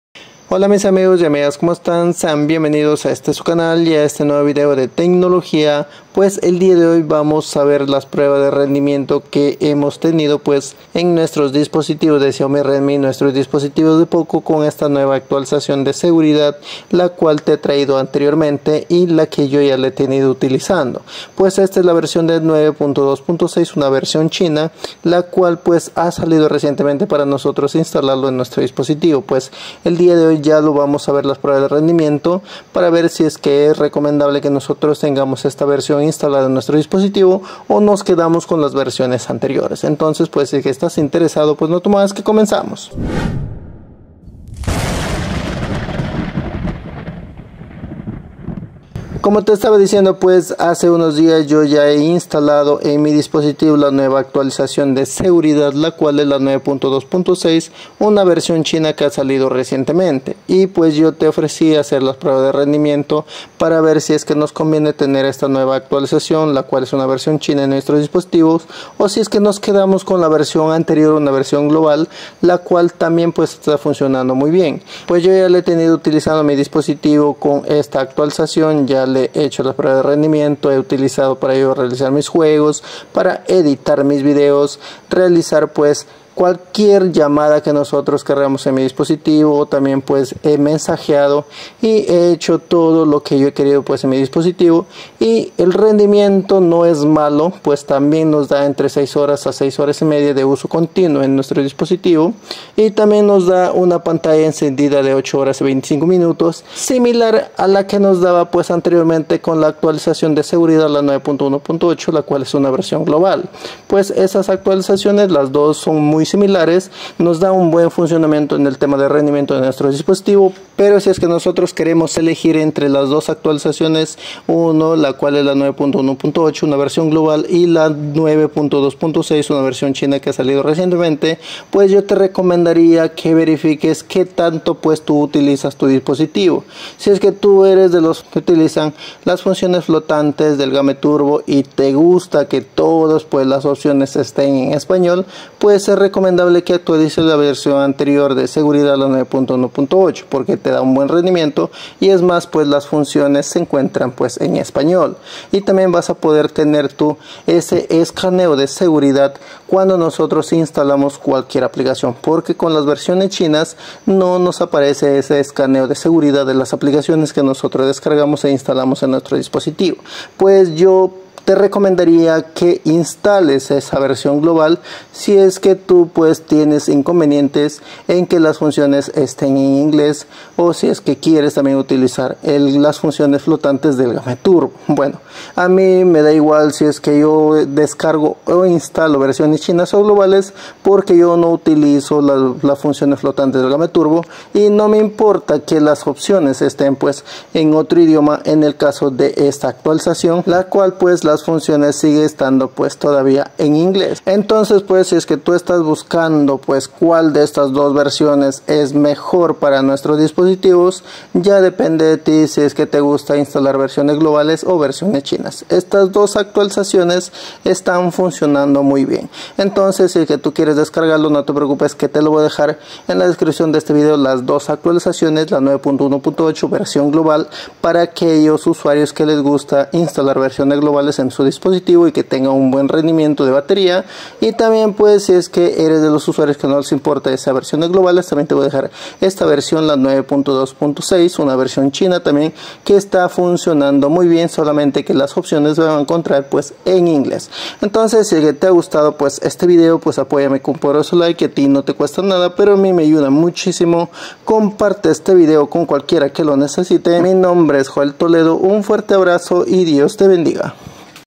🧐¿CONVIENE INSTALAR ESTA SEGURIDAD 9.2.6 EN MI XIAOMI? | PRUEBAS DE BATERÍA
Hola mis amigos y amigas como están, estas son las pruebas de rendimiento en nuestro dispositivo con la actualización de seguridad.

Espero te guste y te sea de mucha utilidad.
Comparte con tus amigos y familiares para que se enteren igual que tú.

Te deseo un excelente día y dios te bendiga

MI GRUPO DE TELEGRAM